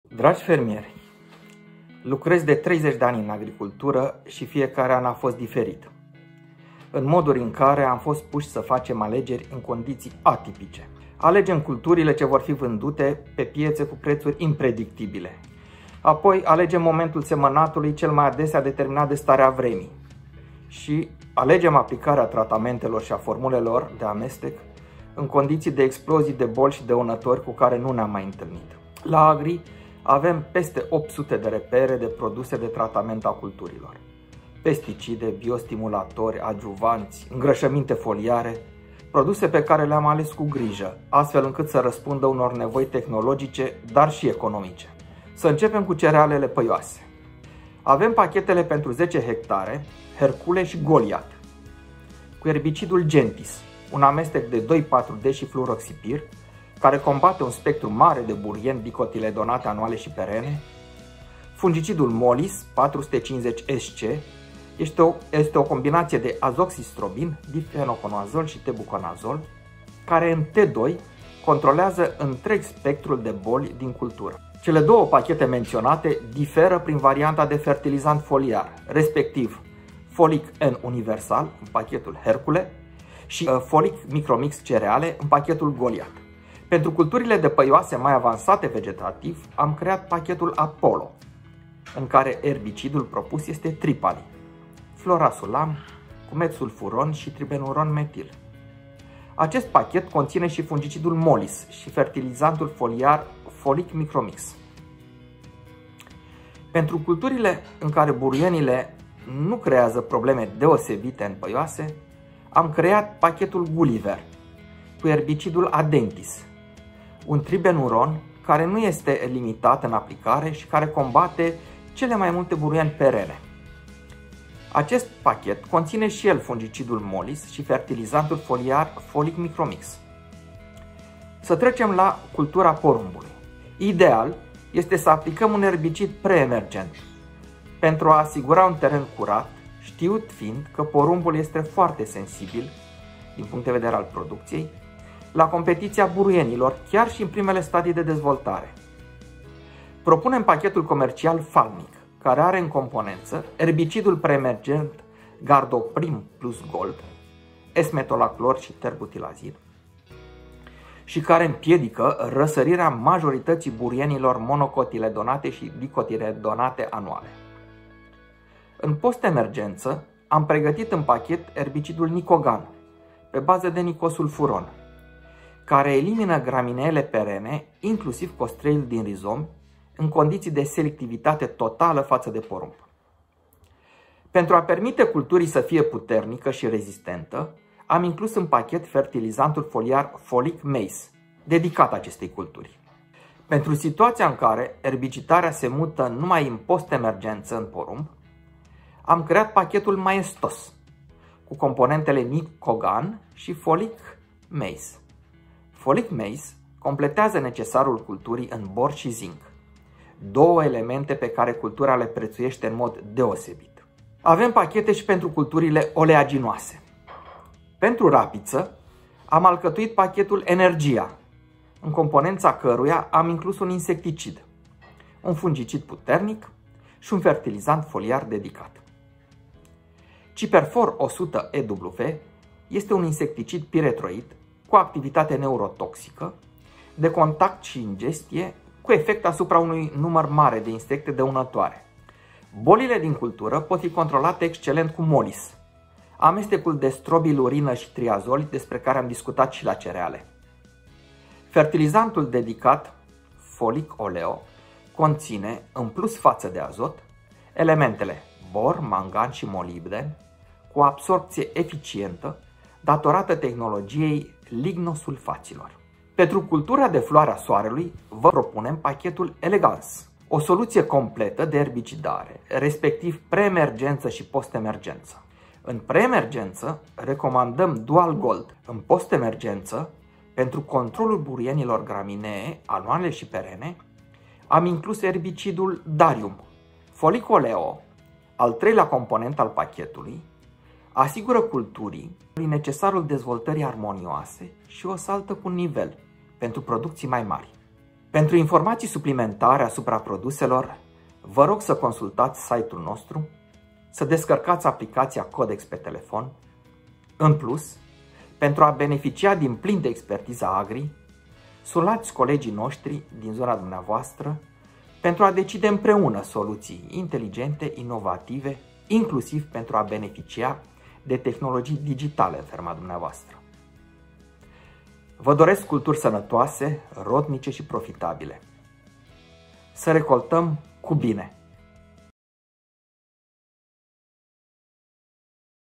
Agrii, furnizorul tău de produse pentru protecția plantelor!
Cristian Grecu, Category Manager Pesticide, Agrii România, ne vorbește despre pachetele comerciale promovate de grup și flexibilitatea acestora.

Ca întotdeauna: Spor la treabă si Recolte Bogate !